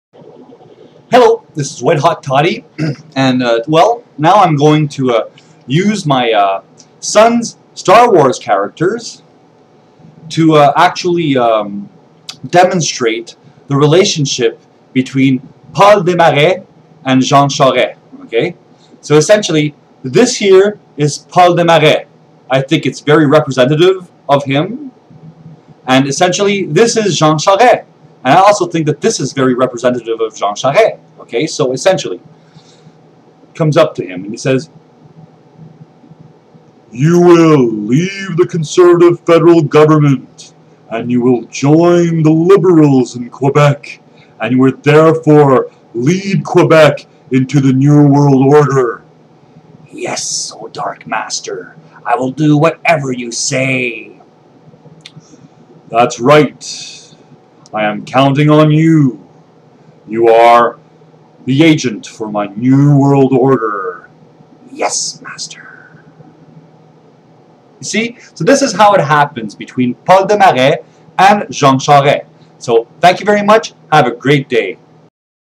WhiteHotToddy`s Parody of The Relationship between Paul Desmarais and Jean Charest
I`ll use my son`s star wars figurines to play out the relation ship between the head of the power corporation and the prime minister of the quebec government.